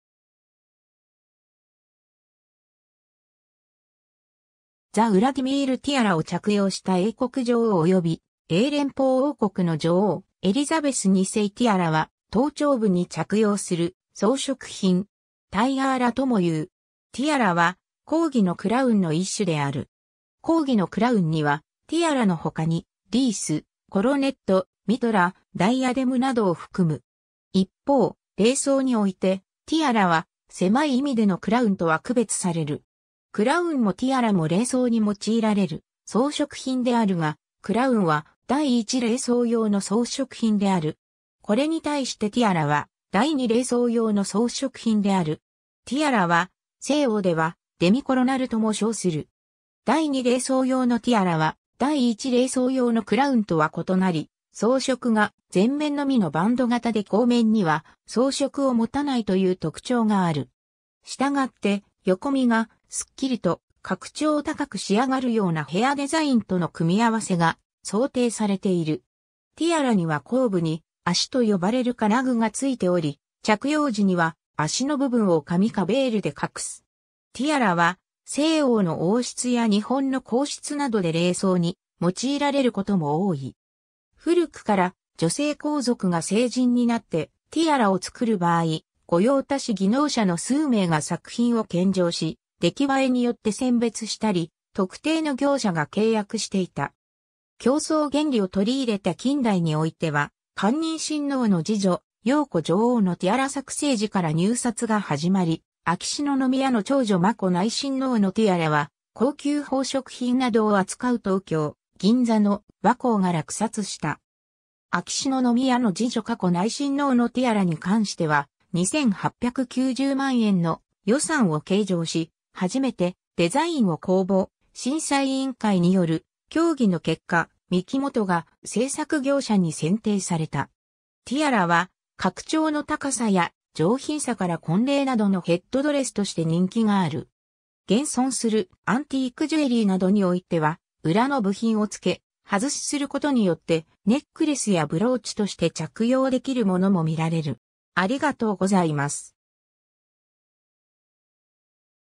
ティアラ
ザ ウラディミール ティアラを着用した英国女王及び英連邦王国の女王・エリザベス2世（1959年） ティアラ (Tiara) は、頭頂部に着用する装飾品（アクセサリー）。
タイアーラともいう。
 ティアラは広義のクラウン（crown、冠）の一種である。
広義のクラウンには、ティアラのほかに、リース (wreath)、コロネット (coronet)、ミトラ (mitre)、ダイアデム (diadem) などを含む。
 一方、礼装において、ティアラは狭い意味でのクラウンとは区別される。
クラウンもティアラも礼装に用いられる装飾品であるが、クラウンは第一礼装用の装飾品である。
これに対してティアラは第二礼装用の装飾品である。
ティアラは西欧ではデミコロナル（半冠）とも称する。
 第二礼装用のティアラは第一礼装用のクラウンとは異なり、装飾が前面（前頭部）のみのバンド型で後面には装飾を持たないという特徴がある。
したがって、横見がすっきりと格調高く仕上がるようなヘアデザインとの組み合わせが想定されている。
ティアラには後部に足と呼ばれる金具がついており、着用時には足の部分を髪かベールで隠す。
 ティアラは西欧の王室や日本の皇室などで礼装に用いられることも多い。
 古くから、女性皇族が成人になってティアラを作る場合、御用達技能者の数名が作品を献上し出来栄えによって選別したり、特定の業者が契約していた。
競争原理を取り入れた近代においては、寛仁親王の次女瑶子女王のティアラ作成時から入札が始まり、秋篠宮の長女眞子内親王のティアラは、高級宝飾品などを扱う東京・銀座の「和光」が落札した。
秋篠宮の次女佳子内親王のティアラに関しては、2890万円の予算を計上し、初めてデザインを公募、「審査委員会」（審査委員は高階秀爾・大原美術館館長、宮田亮平・東京芸術大学長ら3人を予定）による協議の結果、ミキモトが制作業者に選定された。
 ティアラは格調の高さや上品さから婚礼などのヘッドドレスとして人気がある。
 現存するアンティーク・ジュエリーなどにおいては、裏の部品を着け外しすることによって、ネックレスやブローチとして着用できるものも見られる。